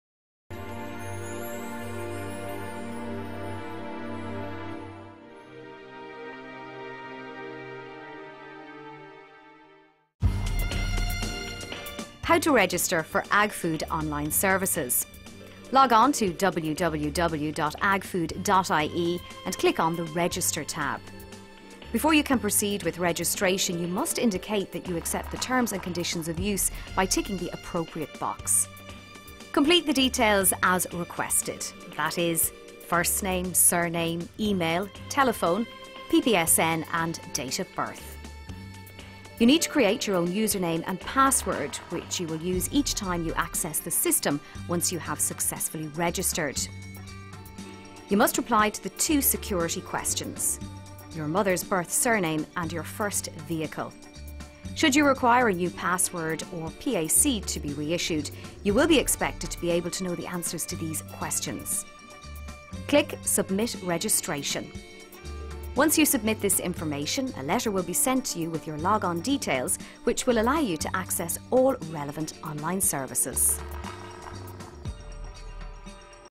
Chapter 03 - How to register for agfood
Chapter 3 of Agfood Online Services Guide - How to register for agfood